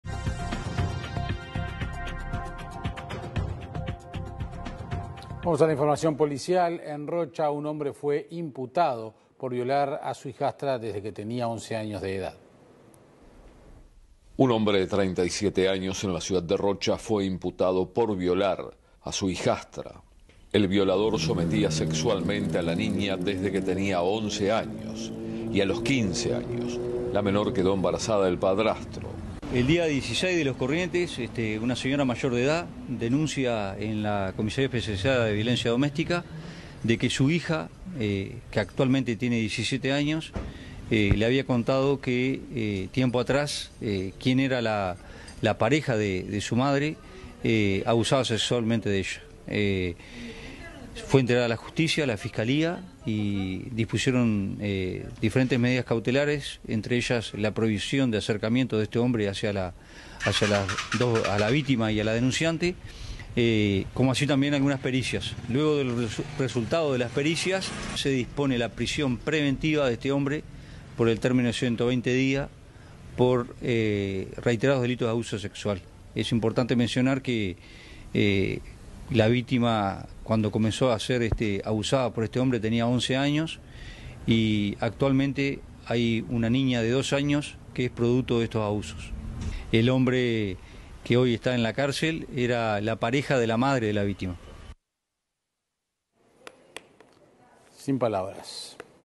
Rocha, un hombre de 37 años violaba a su hijastra desde los 11 años, a los 15 la dejo embarazada
Rocha, un hombre de 37 años violaba a su hijastra desde que tenia 11 años, a los 15 quedo embarazada, el violador esta en prisión preventiva en la cárcel.